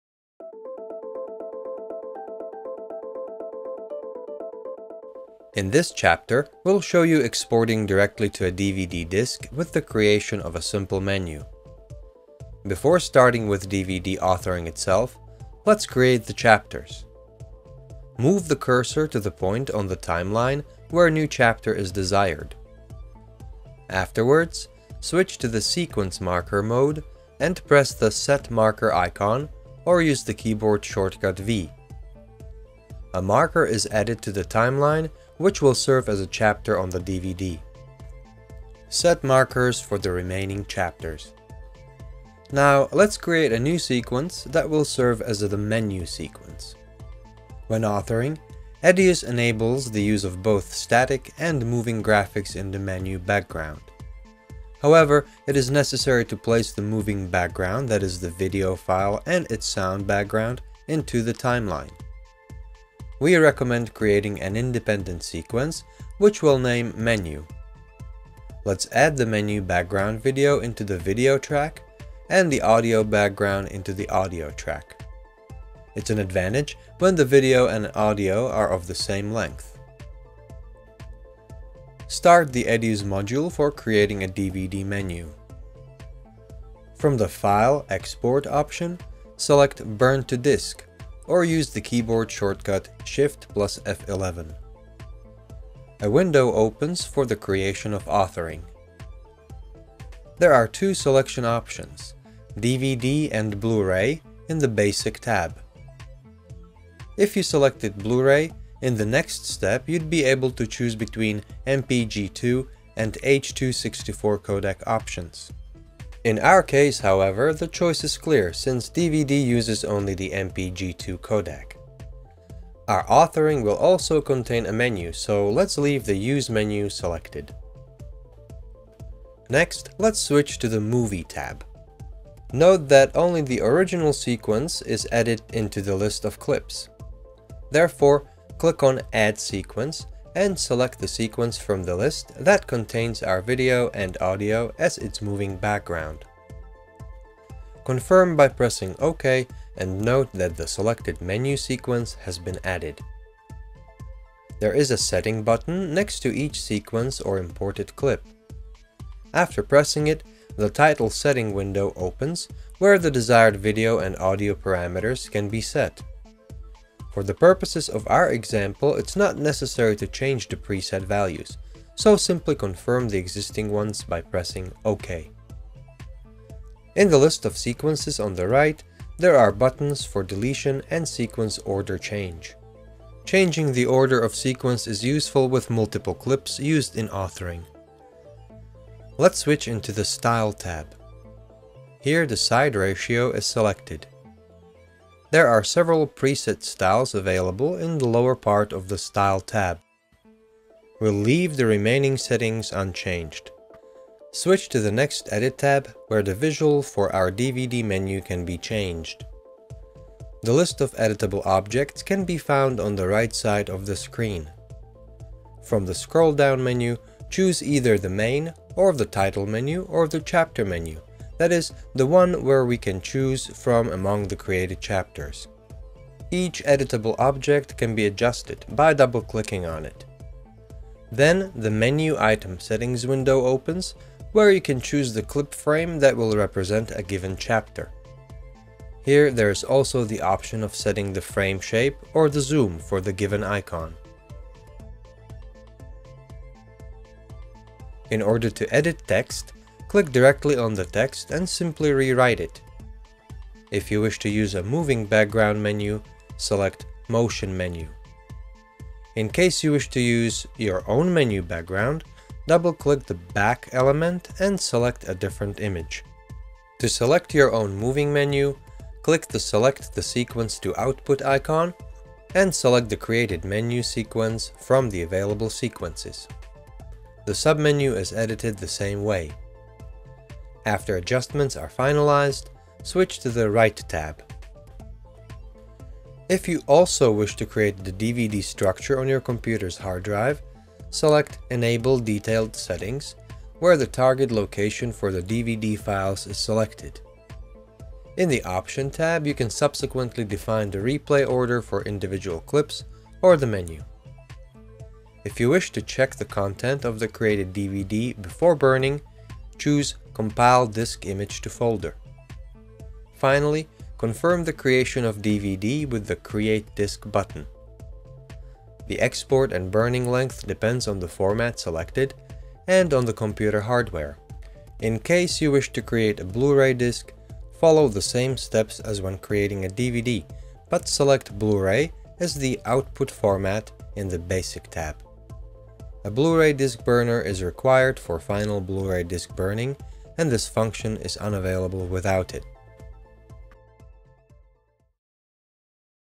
Grass Valley EDIUS Pro - 29) DVD and Blu Ray Authoring
This chapter describes how to export directly to DVD and Blu-Ray, with the creation of a simple menu. The video also focuses on the setting of export parameters and on individual authoring options.